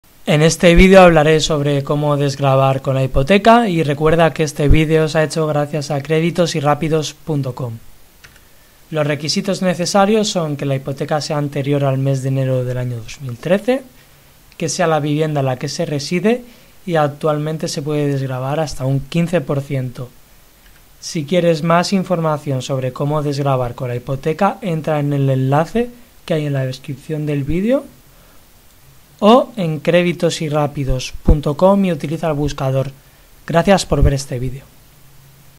Desgravar hipoteca - Requisitos
Gracias a tener una hipoteca antes de enero del 2013, es posible desgravar. Con este vídeo conocerás los requisitos para desgravar las cuotas de una hipoteca.